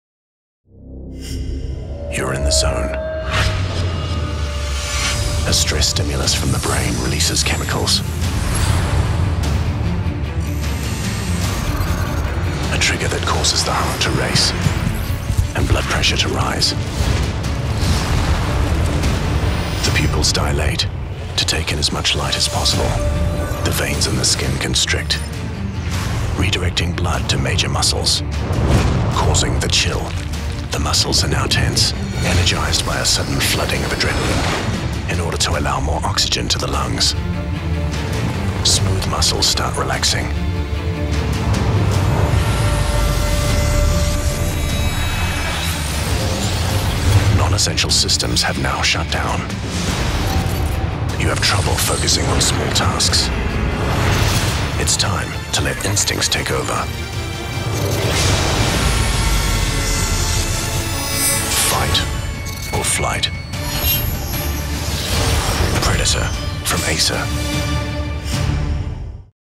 Fight or Flight? The Predators will answer.
"Sie kommen um deinen Stamm zu vernichten... du kannst die Trommeln hören, den stetigen Marsch. Der Feind ist auf dem Kriegspfad.
Du hast nur wenige Sekunden, um deine Entscheidung zu treffen. Drehst du dich um und lässt deinen Stamm zurück, um im Schlamm zu sterben? Oder stellst du dich und kämpfst für deine Brüder und Schwestern, deine Kameraden, deine Freunde, deinen Stamm?
Lass dich von deinen Instinkten leiten.
Mache die Jäger zu den Gejagten.
Trete den Predators bei.
Mache dich bereit für die brandneuen leistungsstarken Geräte der Acer Predator Gaming-Serie.“